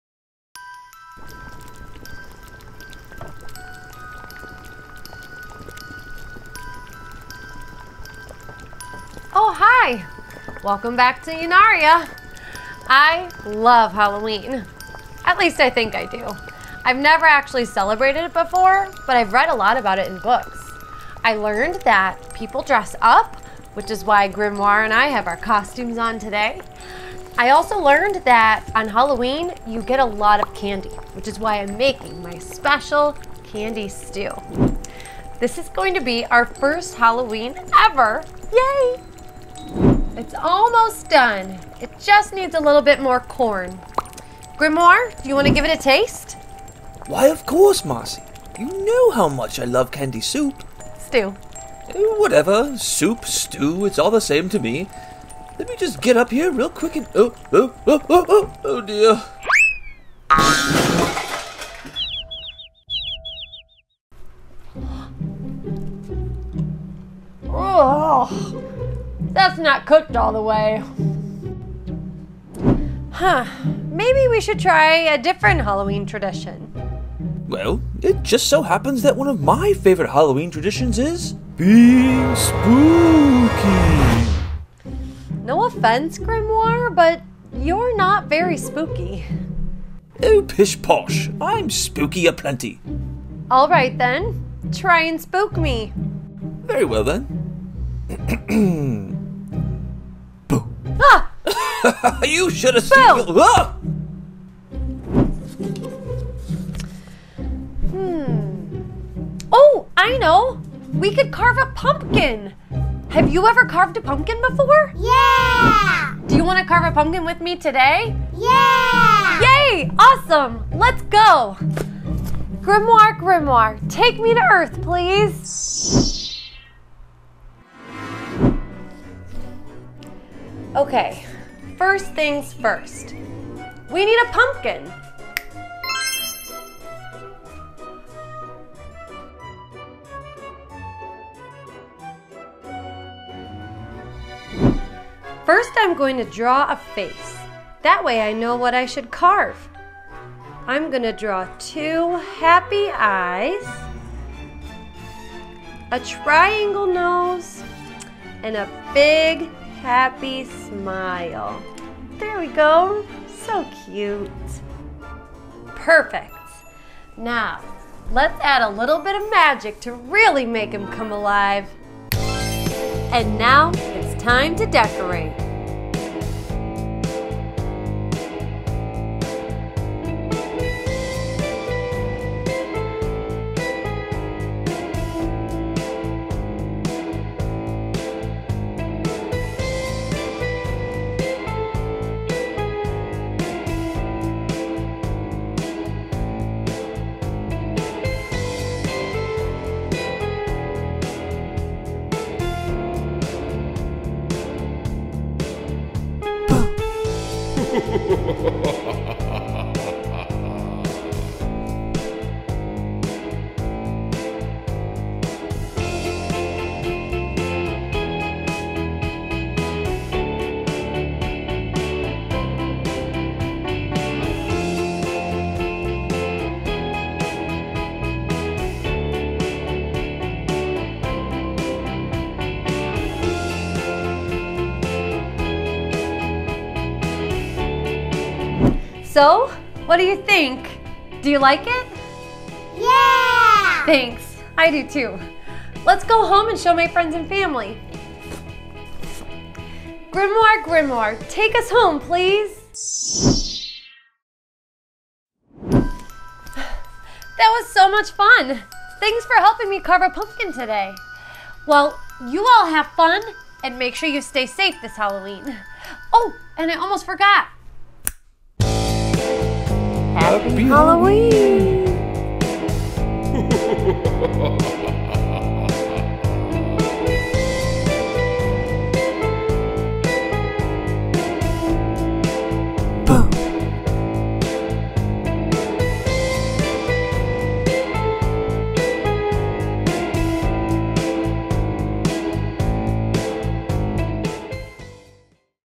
Princess Play Date: Princess Marci Makes A Jack-O'-Lantern | Fun Videos for Kids
Marci is a sorcerer in training, who loves to explore and learn new things - just like YOU!

Happy Halloween!
In this fun video for kids - Halloween is here! Join Princess Marci as she celebrates one of her favorite holidays - Halloween!  In this episode, Marci tries to make some candy soup and carve her very first pumpkin…using magic of course!

Thanks for joining Princess Marci and helping her learn new things! Be sure to be on the look out for more videos for kids and videos for toddlers!

Creative Credits:
SFX: